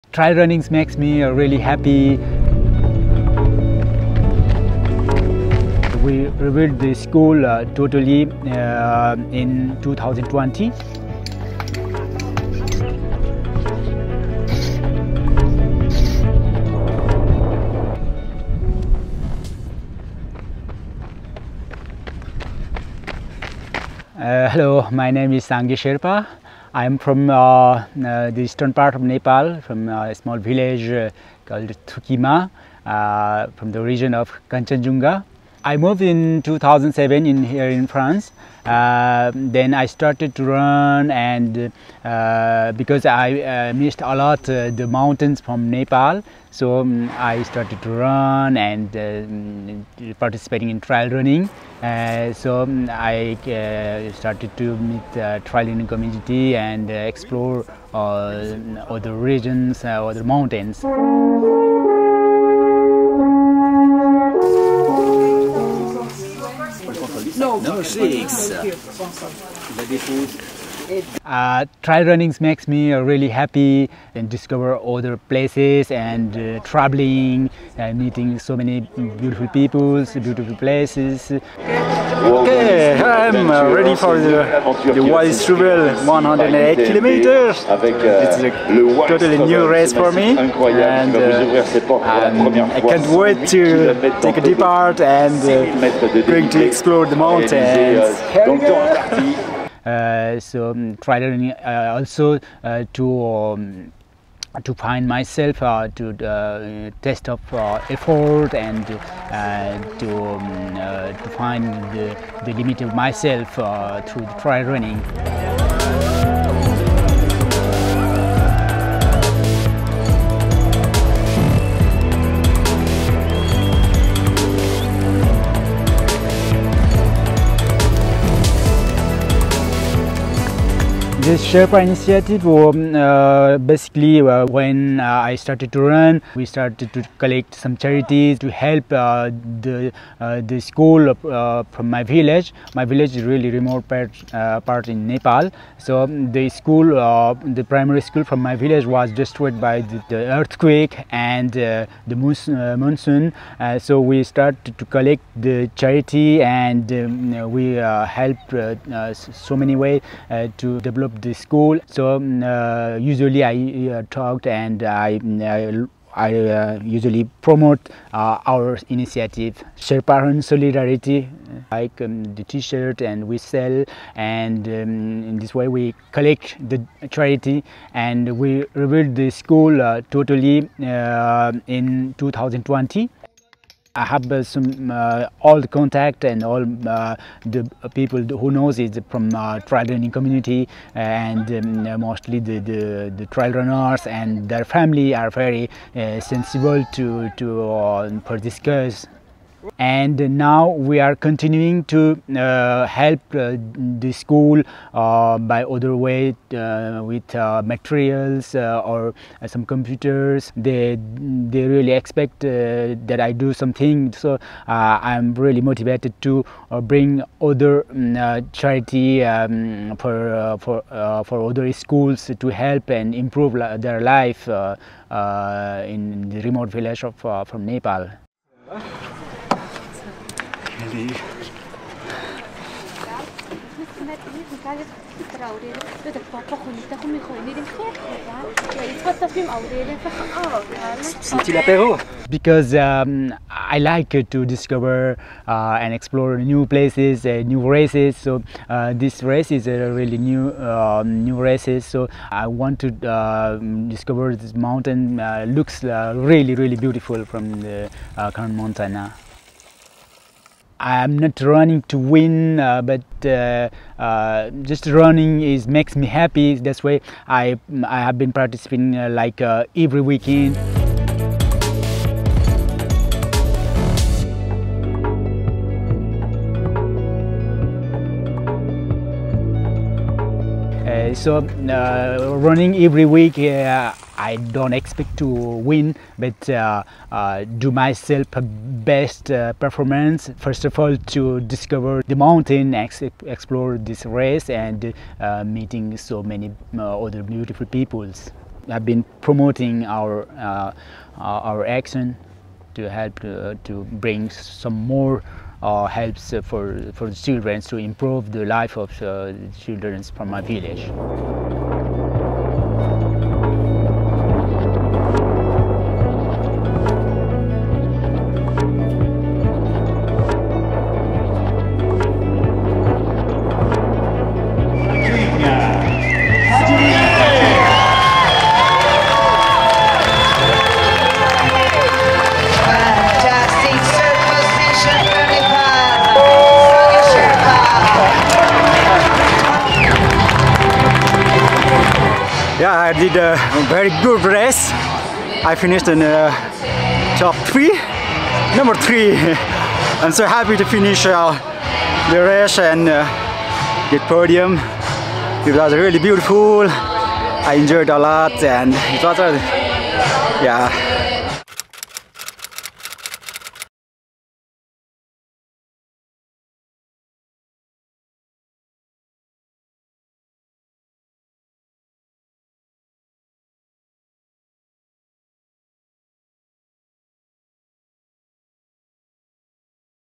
Fund Run｜Inspiring Adventurer
Sange Sherpa is from a remote village in Nepal, who has been doing trail running and raising money to build a school in his village and helping poor children there. We followed him in a 100 KM trail run race on a high mountain in the Alps.